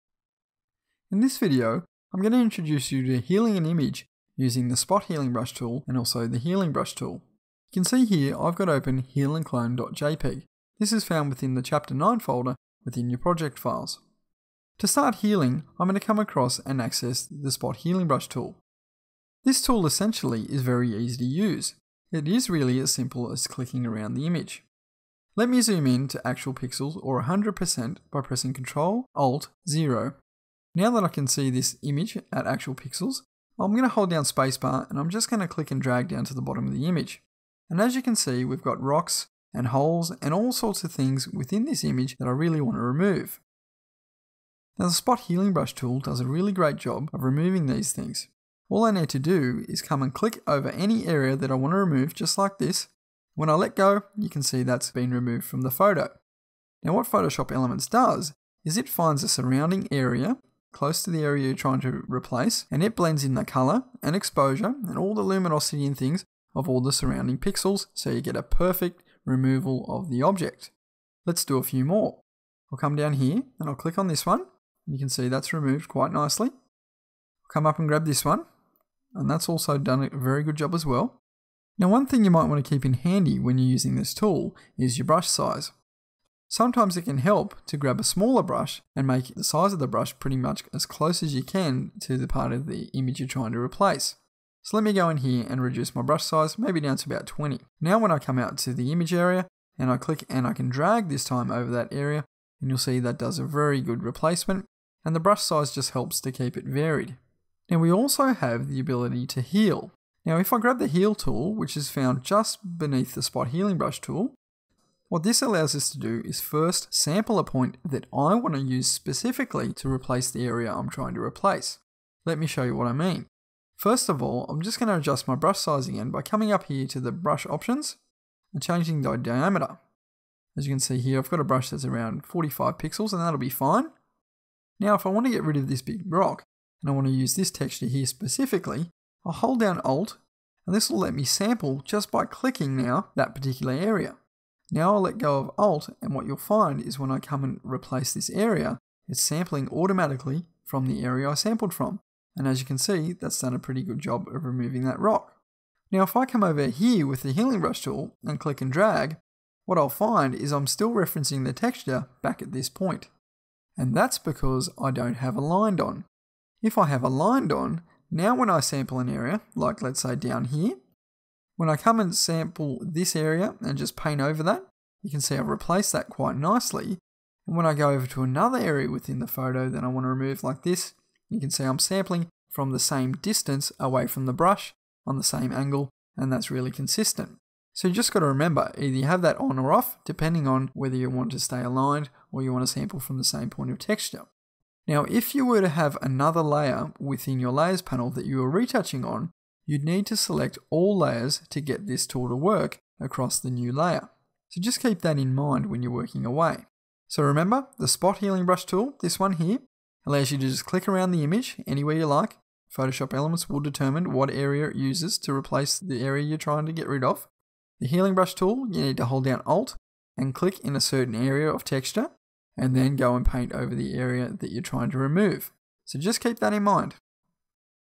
Photoshop Elements 7 Tutorial Video Healing Brush
Want all of our free tutorial videos?

to view this and all the other Photoshop Element 7 videos in high quality. This Adobe Photoshop Elements tutorial shows how to using the healing brush YouTube